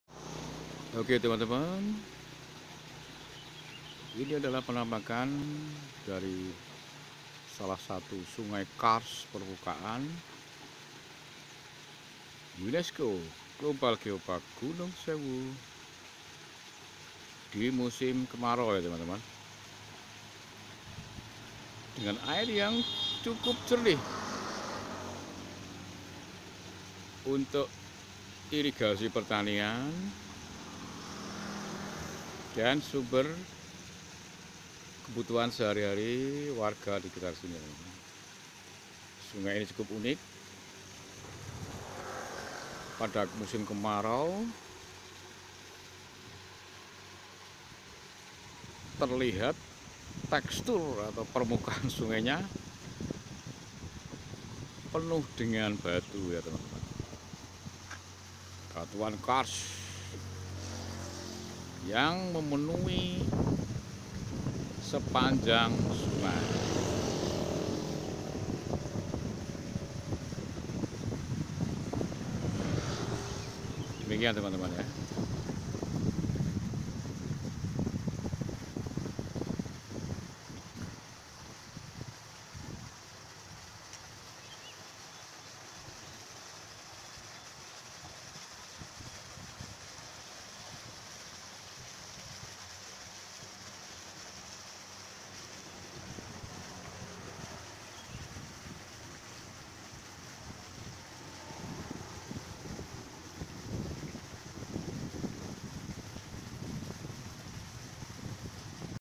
Karst Pacitan Situs Wisata Unik Sungai Batu Kebonagung Geopark Gunung Sewu
Adalah penampakan salah satu sungai karst permukaan yang cukup unik..berada di UNESCO Global Geopark Gunung Sewu Area Pacitan